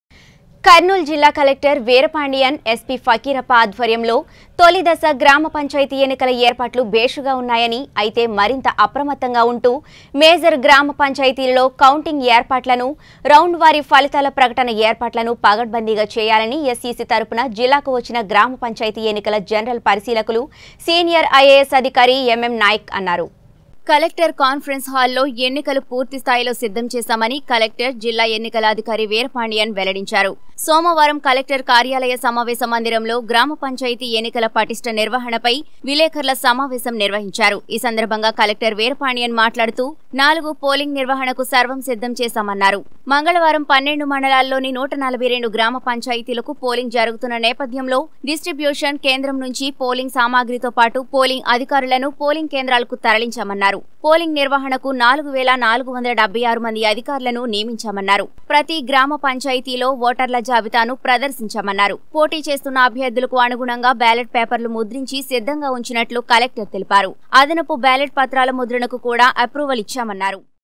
తొలిదశ పోలింగ్, కౌంటింగ్ ఏర్పాట్లు పకడ్బందీగా చేయండి కలెక్టర్ ఆదేశాలు - కర్నూలు జిల్లా
తొలిదశ పోలింగ్, కౌంటింగ్ ఏర్పాట్లు పకడ్బందీగా చేయండి కలెక్టర్ ఆదేశాలు - కర్నూలు జిల్లా | 19 Newstelugu